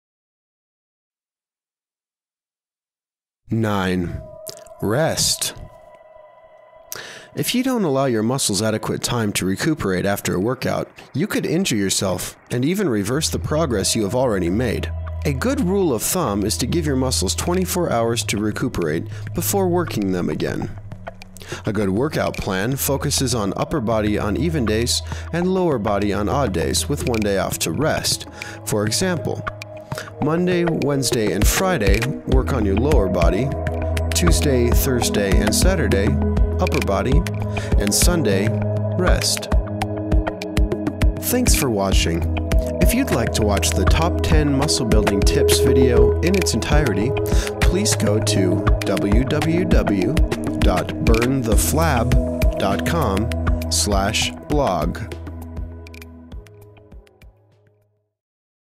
Burning Fat Exercise - Top 10 Muscle Building Tips 09 - Rest
Hey, fatty!  Are you sick of trying to lose that flab, but just can't seem to work it off?  Here are some great muscle building tips to help you blast off that fat!  right now!